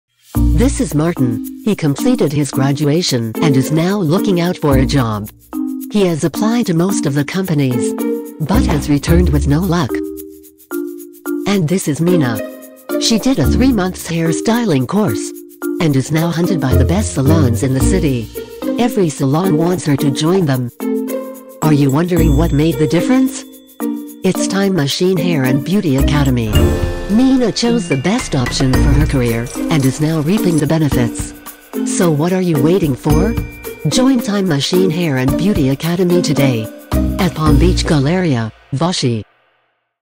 Discover the best Hair, Makeup, Beauty & Nail Academy in Navi Mumbai: Time Machine Academy
Welcome to Time Machine Academy, the premier destination for those looking to start a career in Hair, Makeup, Beauty & Nails in Navi Mumbai. Our Academy is committed to providing top-notch education and training to all of our students, equipping them with the skills and knowledge they need to succeed in the industry.

Our experienced instructors have a wealth of knowledge and expertise in their respective fields and are dedicated to helping our students achieve their goals. Whether you're looking to start your own business, work in a salon, or pursue a career in the entertainment industry, we've got you covered.

At Time Machine Academy, we offer a wide range of courses designed to meet the needs of students at all levels, from beginners to advanced learners. Our state-of-the-art facilities and hands-on training approach ensure that our students receive the best possible education and are prepared for success in the real world.

Don't wait any longer to start your journey towards a fulfilling and rewarding career in Hair, Makeup, Beauty & Nails.